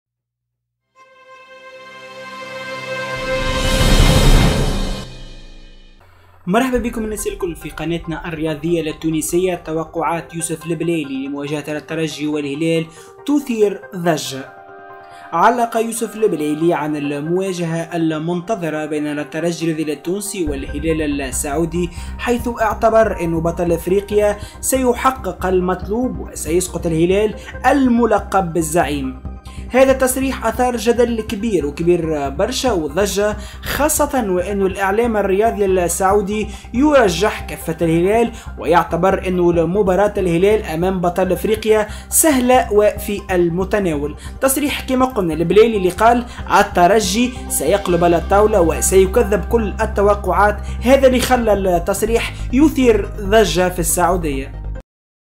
توقعات يوسف البلايلي لمواجهة الترجي والهلال تثير ضجة
لمزيد الاخبار الرياضية زورو موقعنا الرسمي

علق يوسف البلايلي على المواجهة المنتظرة بين الترجي والهلال حيث اعتبر ان بطل افريقيا سيحقق المطلوب ويُسقط الهلال الملقب بـ«الزعيم»، هذا التصريح اثار جدلا وضجة خاصة وان الاعلام الرياضي السعودي يرجح كفة الهلال ويعتبر ان مباراة الهلال امام بطل افريقيا سهلة وفي المتناول.